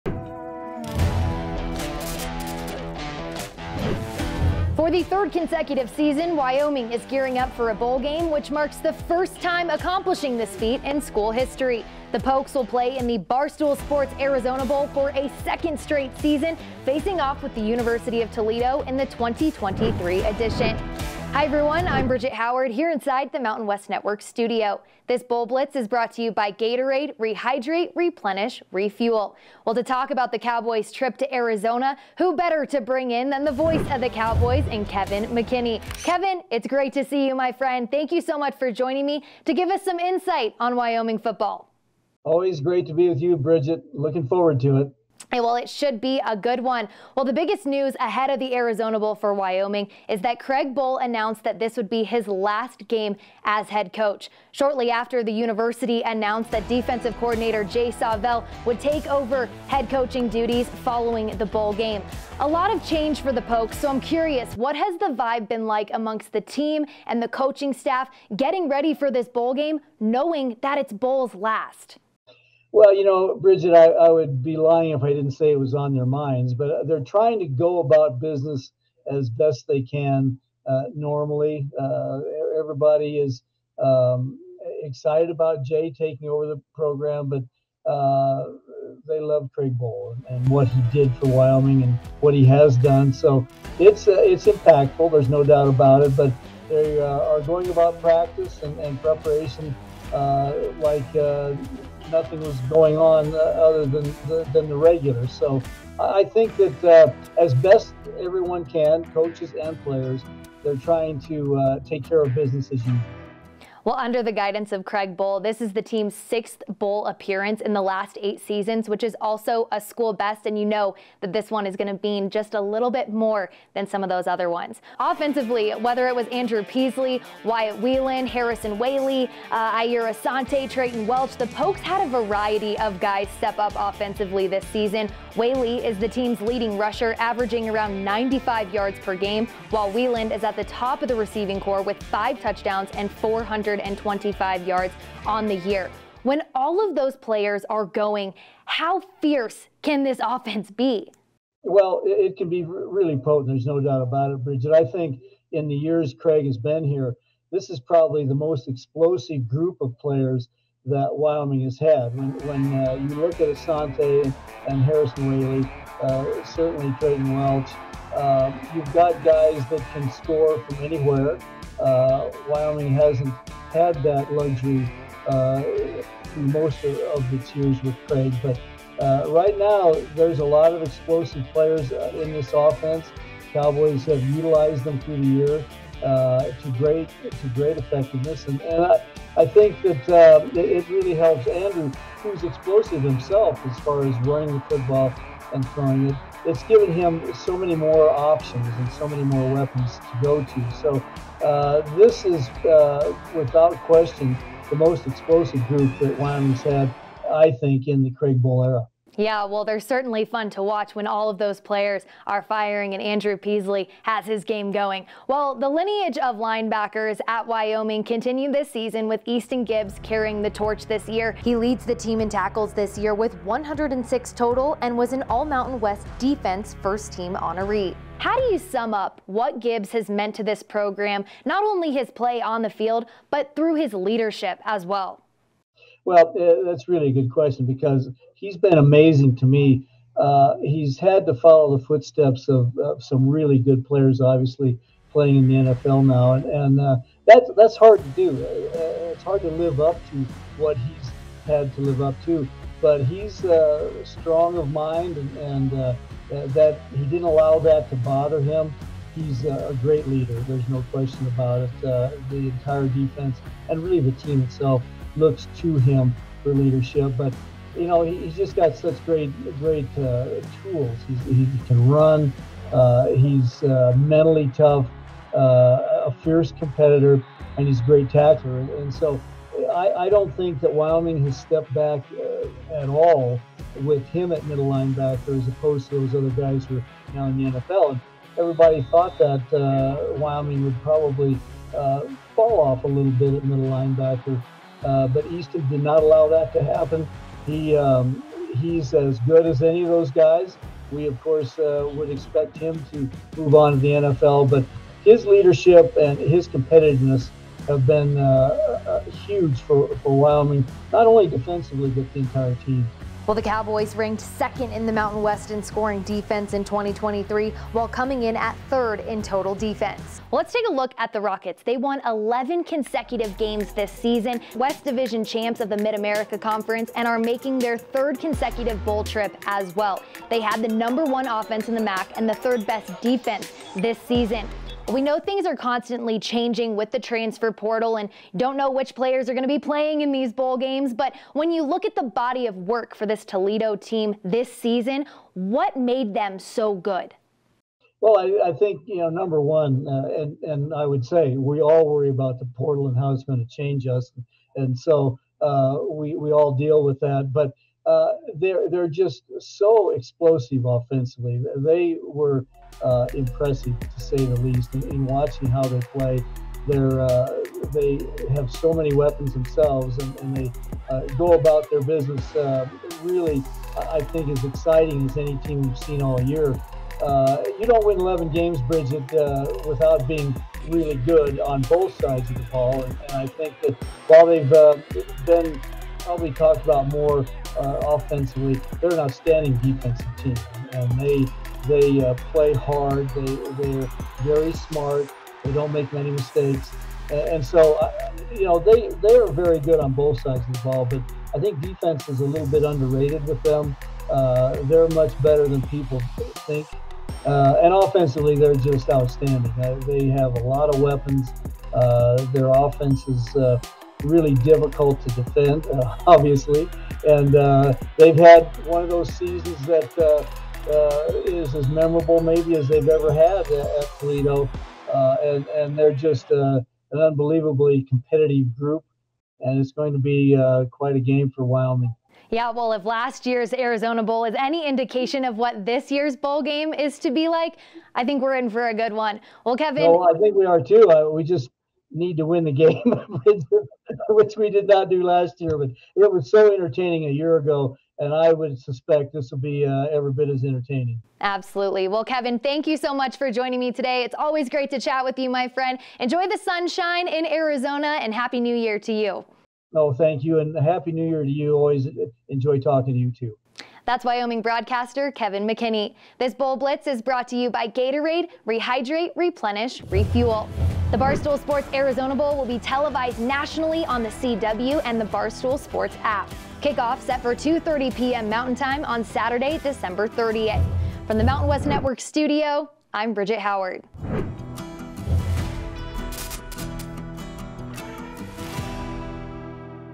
Bowl Blitz 2023: Barstool Sports Arizona Bowl
MWN’s Bridget Howard talks with the voice of the Cowboys Kevin McKinney about Wyoming’s upcoming matchup with the Toledo Rockets in the 2023 Barstool Sports Arizona Bowl.